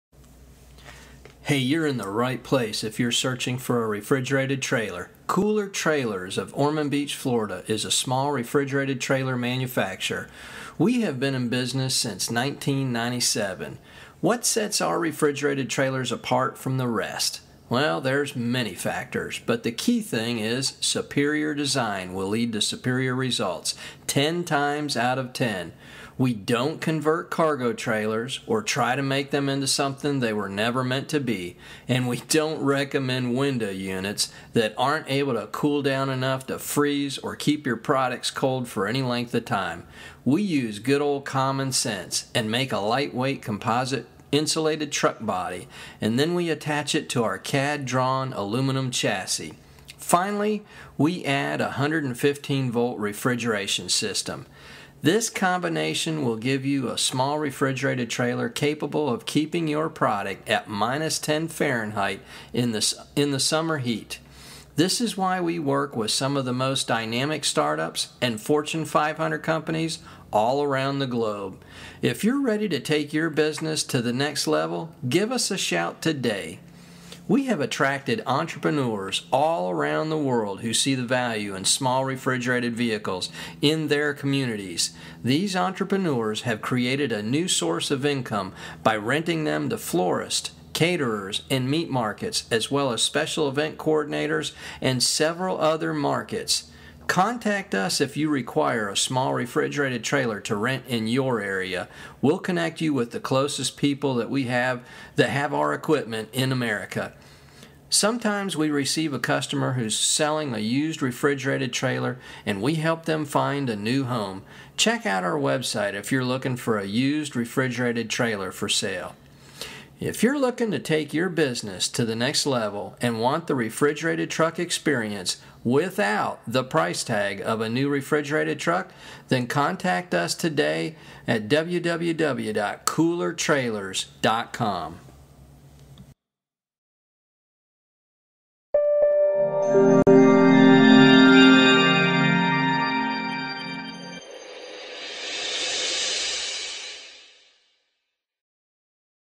Refrigerated Trailer
Your new refrigerated trailer is here! We know you're looking for a high-quality, well designed unit that can withstand decades of use. That's why Cooler Trailers build only the best and our 115 volt refrigeration unit will freeze or cool with any 15 amp household outlet .

Refrigerated Trailer - Cooler Trailers - Small Refrigerated Trailer

00:00 - Intro Refrigerated Trailer
01:29 - Small Refrigerated Trailer Rental
01:55 - Used Refrigerated Trailer
02:15 - Refrigerated Truck Alternative